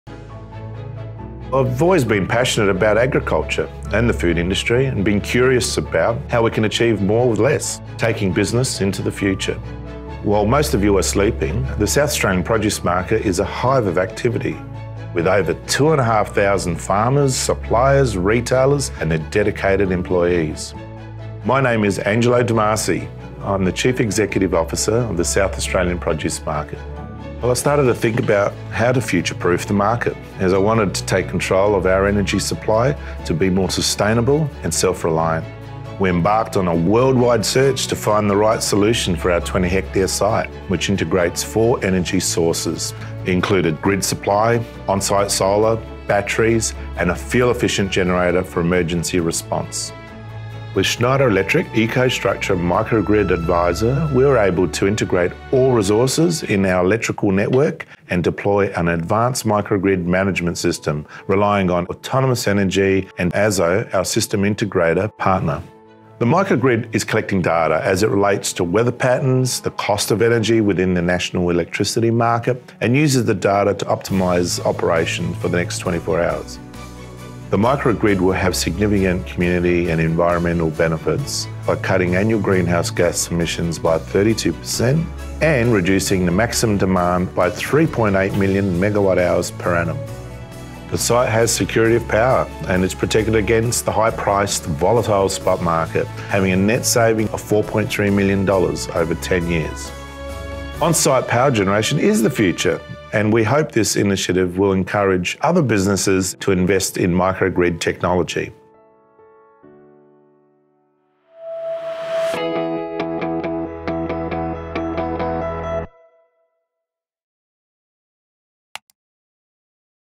EcoStruxure Improves Reliability for the South Australian Produce Market (SAPM) | Schneider Electric
Schneider Electric’s EcoStruxure Microgrid Advisor improves reliability and sustainability for the South Australian Produce Market (SAPM).

By integrating and optimising all resources in SAPM’s electric network, SAPM is able to generate cost savings and reduce greenhouse gas emissions. With a 20-hectare site and four energy sources including grid supply, onsite solar, batteries and a fuel-efficient generator, see how SAPM takes control of energy to become more sustainable and self-reliant.